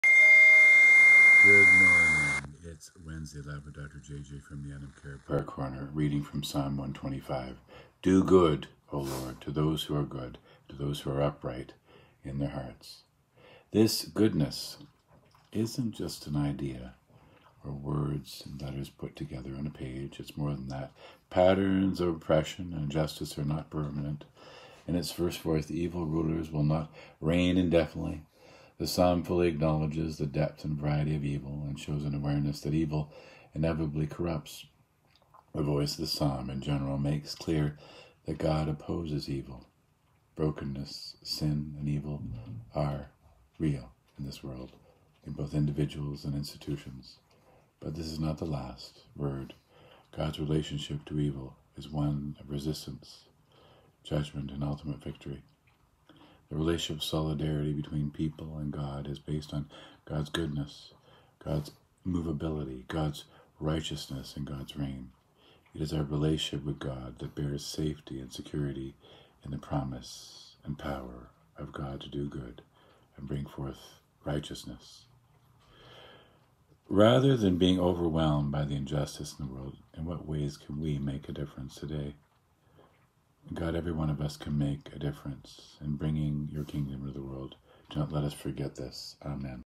Live with Dr Jj
Good morning it’s Wednesday live with Dr Jj from the Anam cara prayer corner